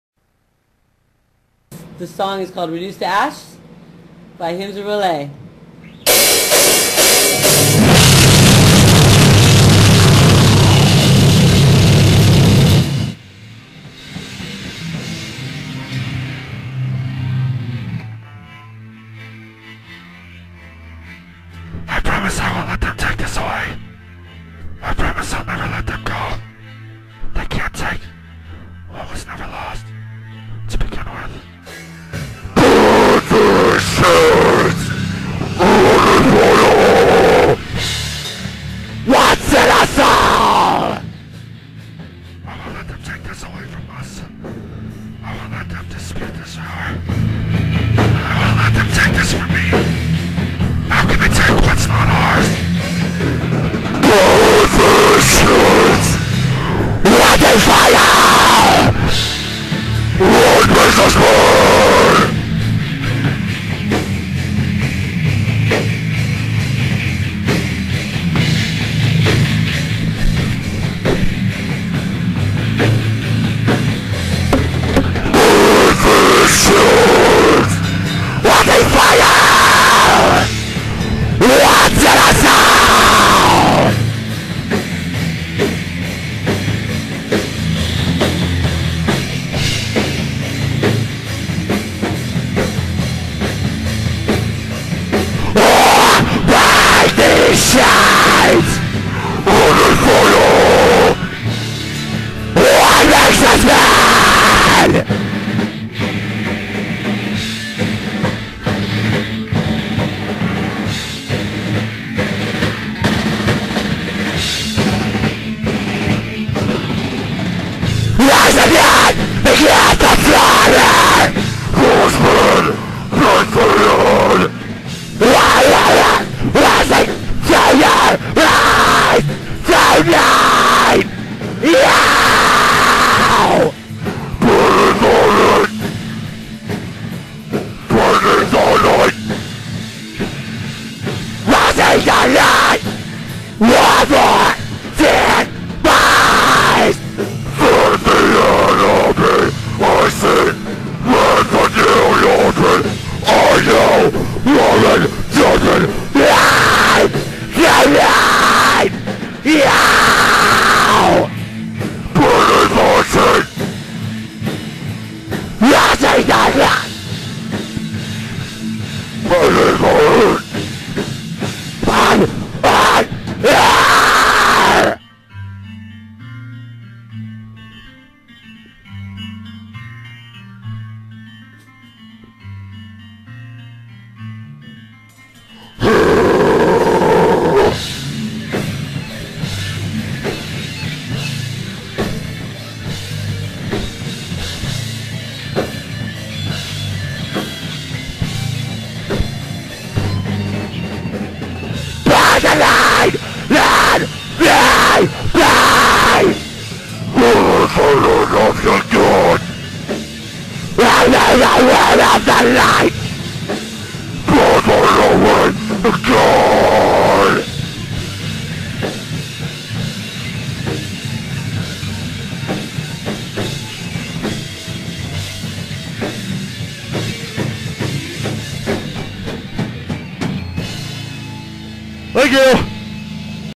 hor1.wmv
fresno metal band hymn of rlyeh on tower t.v. friday aug. 13th, 2010